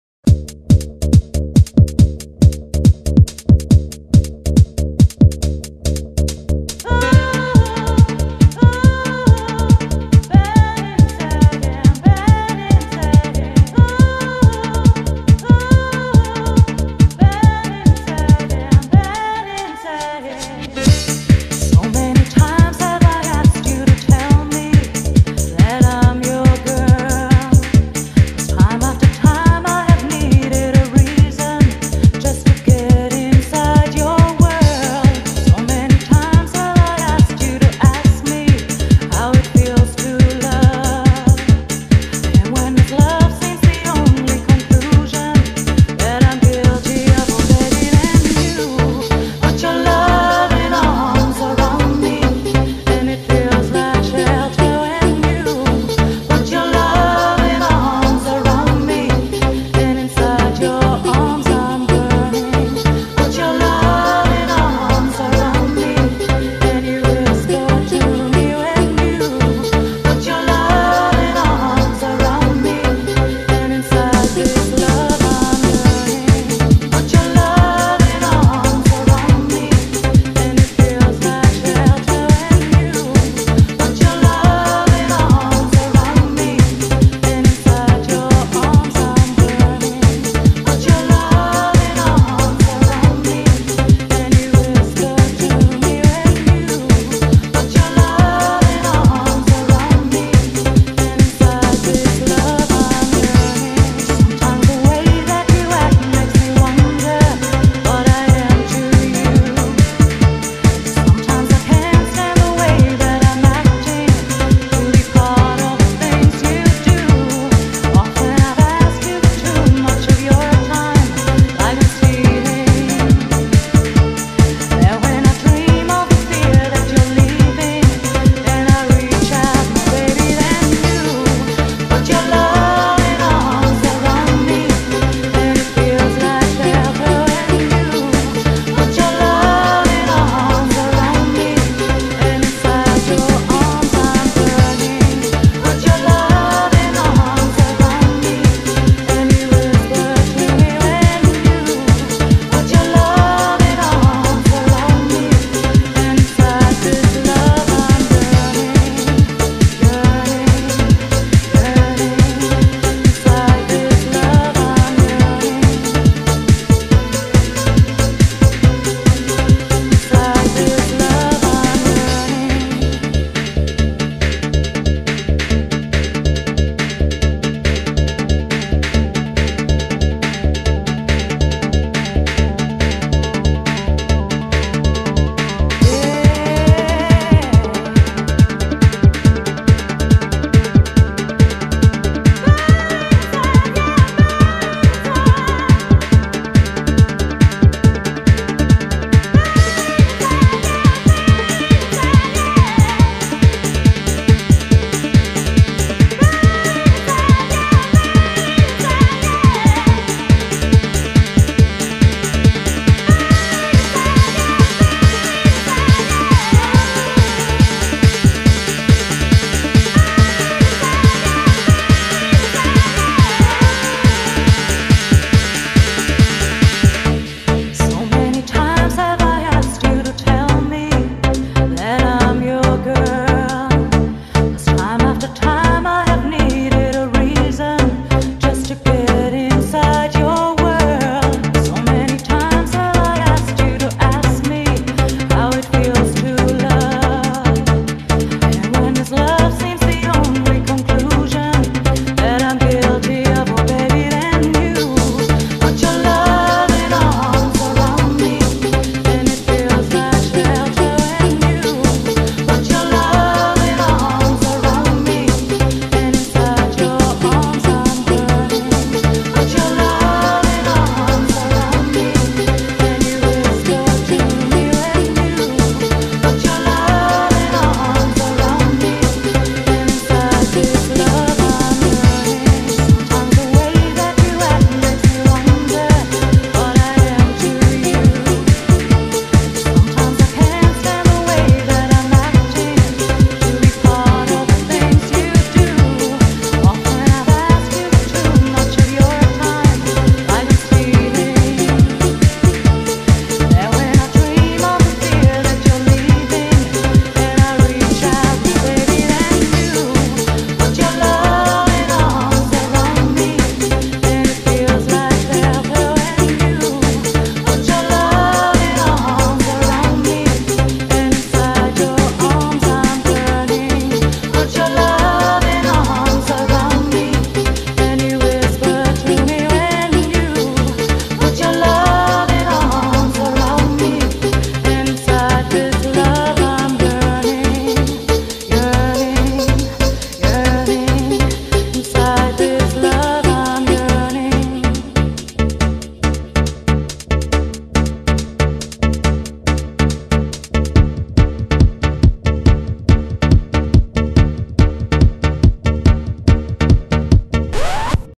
Paul Van Dyk vs Billie Ray Martin
real blast from the past this time. Two classic tracks that fit really well together. Mixed this many years ago so the video may not be perfect!